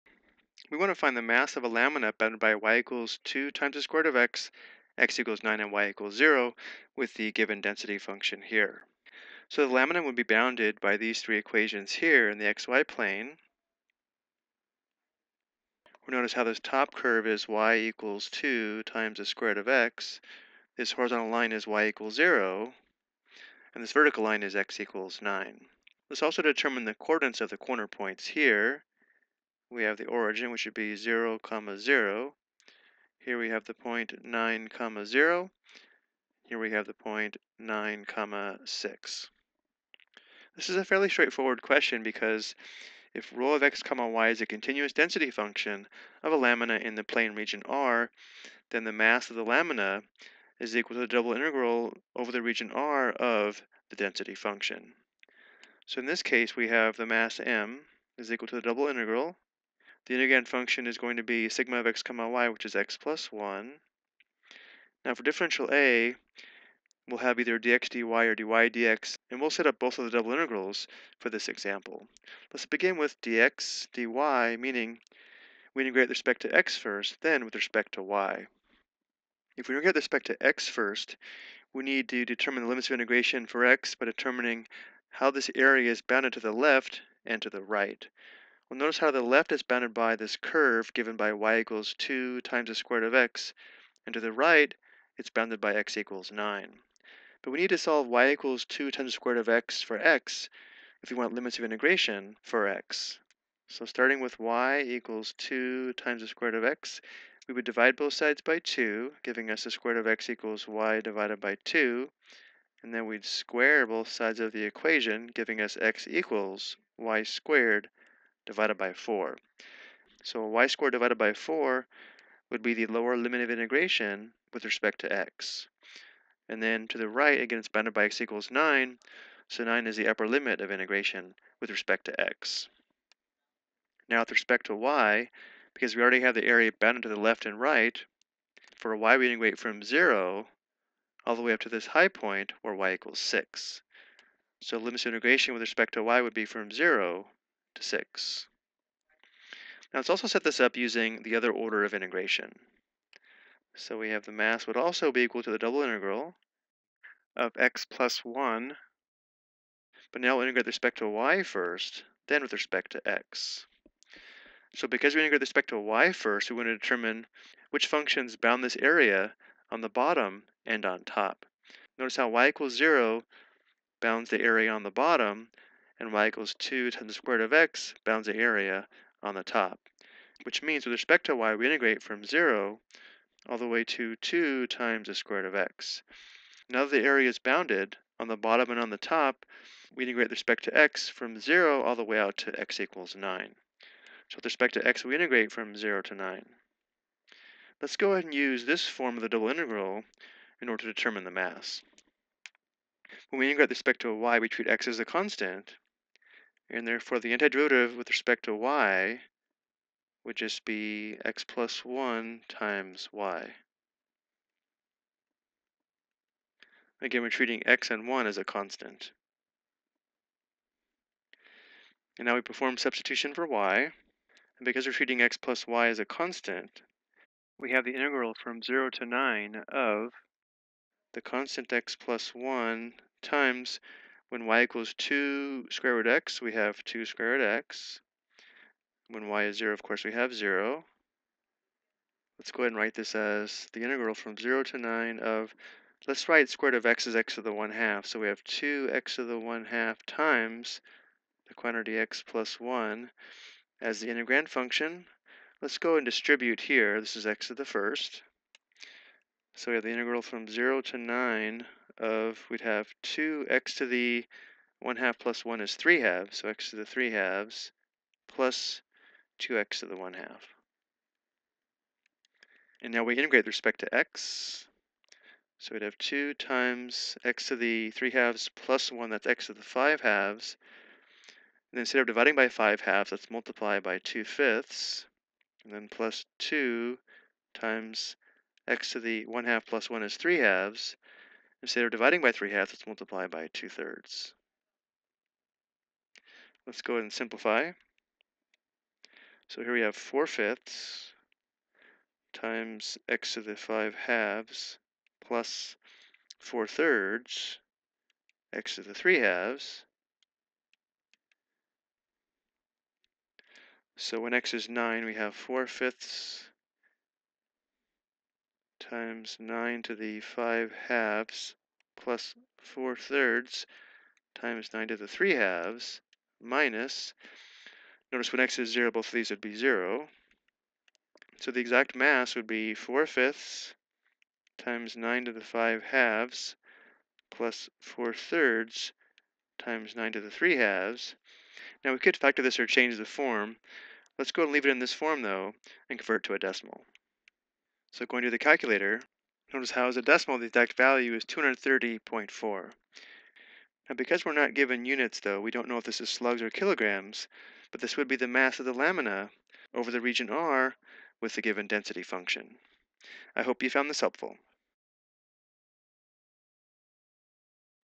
Double Integrals - Find the Mass of a Lamina Over a Region in the xy Plane
This video explains how to set up and evaluate a double integral to determine the mass of a lamina given the density function.